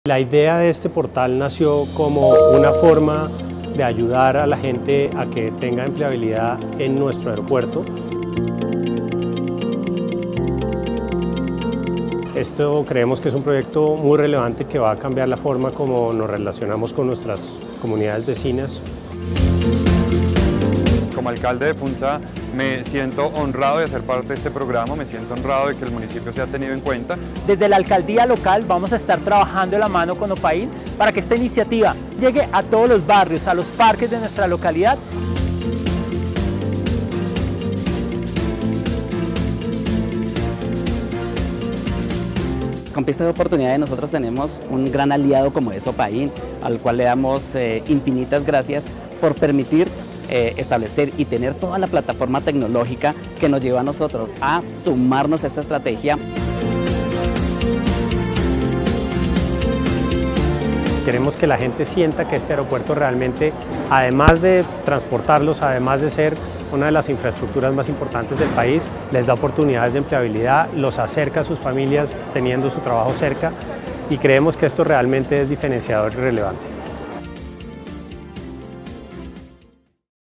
PISTA DE OPORTUNIDADES
Las oportunidades para nuestros vecinos ya tienen una pista donde aterrizar ✈️

🤝💻 Pista de oportunidades es una plataforma digital que lleva una bolsa de ofertas laborales a las comunidades de Engativá, Fontibón y Funza. Gracias a las empresas de carga, aerolíneas y comercios del aeropuerto, que ponen sus vacantes en la plataforma. 🔝✈️🏡
¡Bienvenido al primer portal de empleabilidad del sector aeroportuario!